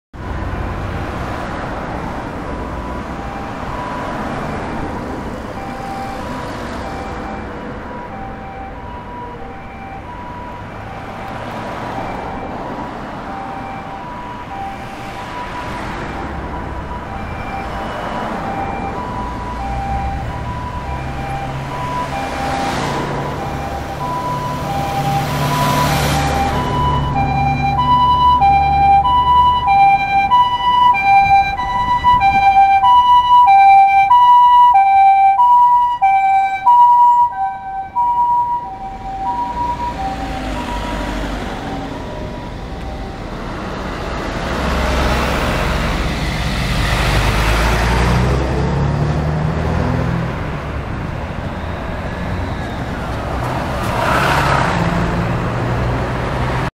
明石市 救急車 緊急走行 テイク9 国道2号線からマンションへ進入
2009年12月撮影。兵庫県明石市大久保付近。大久保分署の救急車です。
※救急車の来る少し前から撮影しています。しばらくお待ち下さい。
※救急車の動きは、無線傍受等何の予備情報も持っていなかった撮影者の想定外の結果になりました。